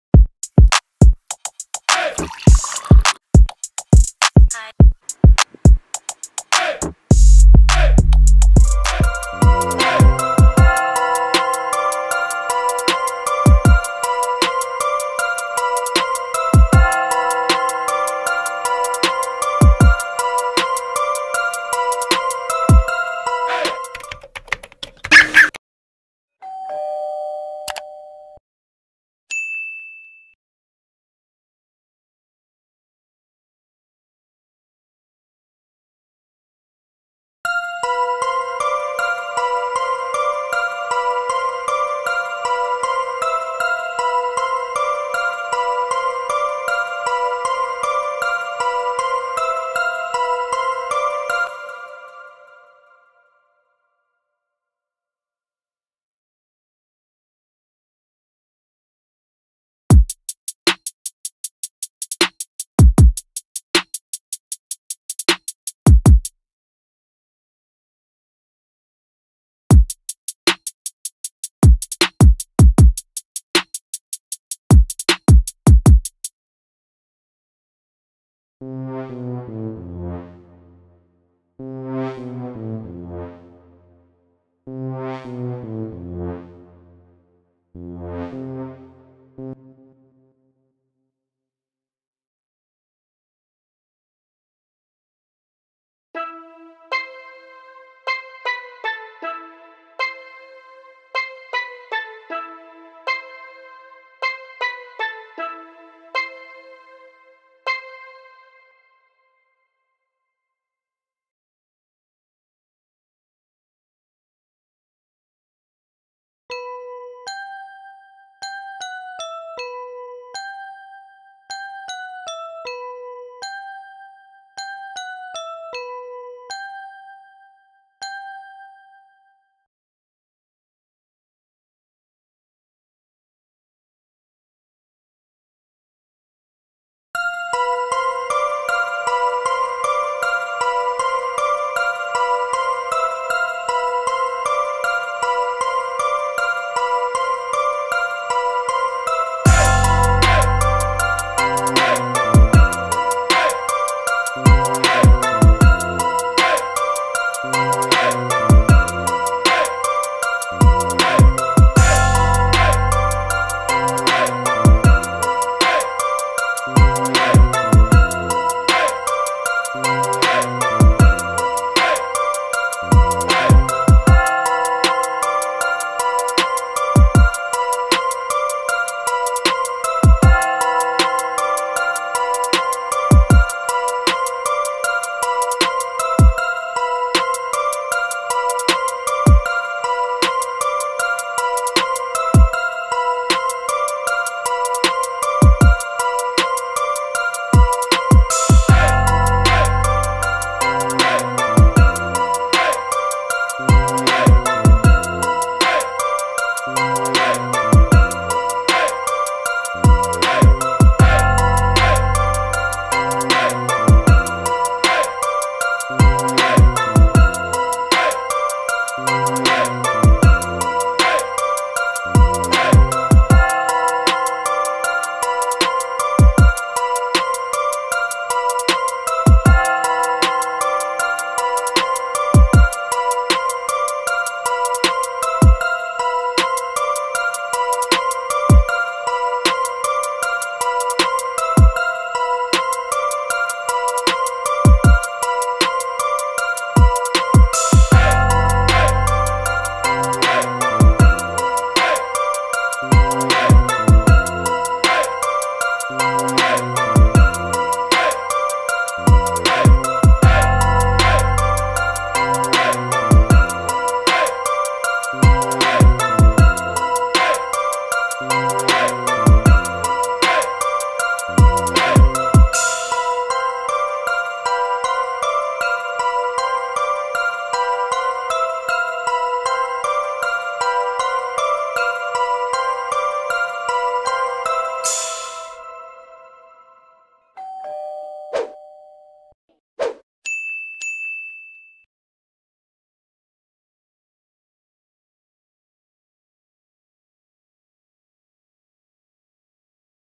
Making Trap Hip Hop Beat in FL Studio - Simple Beat Instrument
This beat I made it with stock plugin fl studio, Vst fl studio and some packs hip hop. I will review this beat.

This video made with :
B.I Entertainment
Half Past Studio